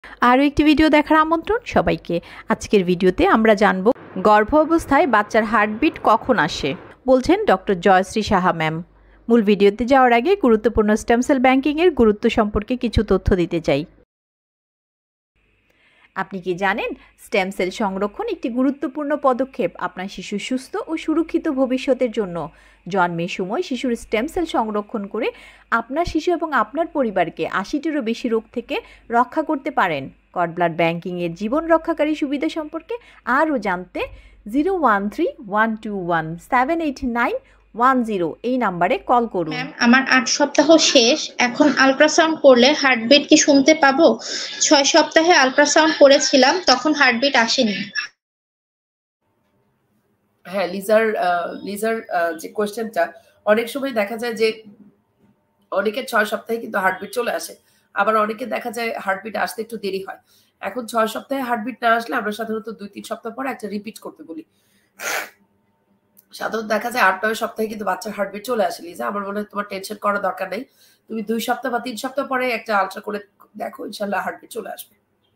👉গর্ভাবস্থায় বাচ্চার হার্টবিট কখন আসে ?
👉জন্মের সময় আপনার শিশুর স্টেম সেল সংরক্ষণ করে আপনি আপনার পরিবার কে ৮০+ রোগ থেকে আজীবন সুরক্ষিত রাখতে পারবেন।

👉কর্ডব্লাড ব্যাংকিংয়ের জীবন রক্ষাকারী সুবিধা সম্পর্কে আরও জানতে আজই আমাদের সঙ্গে যোগাযোগ করুন এই নম্বরে: ০১৩১২১৭৮৯১০